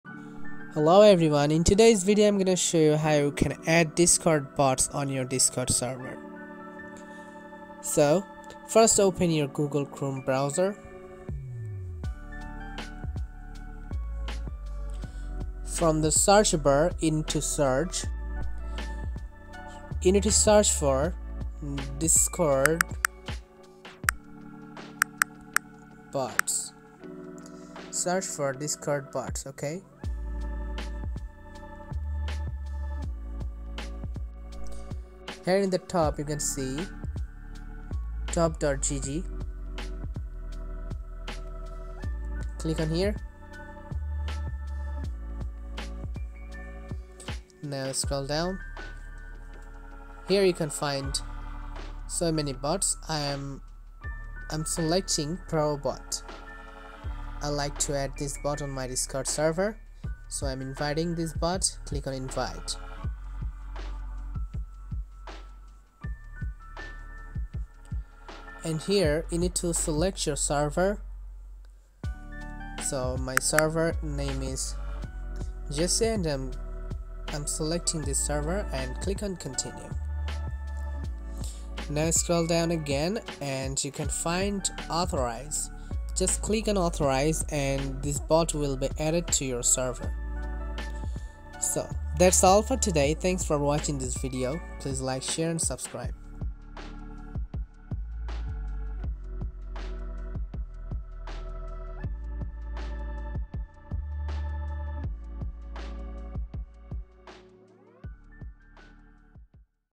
How to Add a DISCORD BOT to a Discord ServER
In this video, I'll be SHOWING you how to add a Discord bot to a Discord server and going over how to make sure your permissions are set correctly so your Discord bots will work properly.